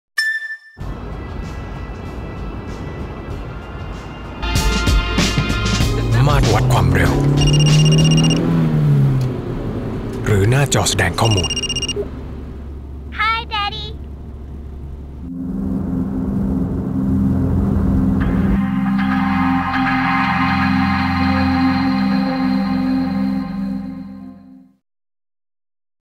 All-new Honda Civic กับระบบ Driver Information Interface ให้คุณไม่ต้องละสายตาจากการขับขี่
ในปัจจุบันเทคโนโลยีด้านยานยนต์ถูกพัฒนา และออกแบบให้ผู้ขับได้รับความรู้สึกประดุจเป็นหนึ่งเดียวกับตัวรถมากยิ่งขึ้น เห็นได้ชัดจากหลักการออกแบบให้ผู้ใช้เป็นศูนย์กลาง ที่มาพร้อมฟังก์ชั่นอำนวยความสะดวกสบายที่ครบครัน สามารถสั่งการระบบต่างๆ ในการใช้งานได้อย่างคล่องตัว เพื่อป้องกันและช่วยลดอุบัติเหตุที่อาจจะเกิดขึ้นจากการที่ต้องละสายตาเพื่อมาสั่งการระบบต่างๆ ของตัวรถ และจะดีกว่าไหม ถ้ารถที่คุณขับ มาพร้อมกับหน้าจอแสดงผลข้อมูลที่มาตรวัด เพื่อให้คุณไม่ต้องละสายตา และเสียสมาธิในการขับขี่ เช่น ระบบ Driver Information Interface ใน All-new Honda Civic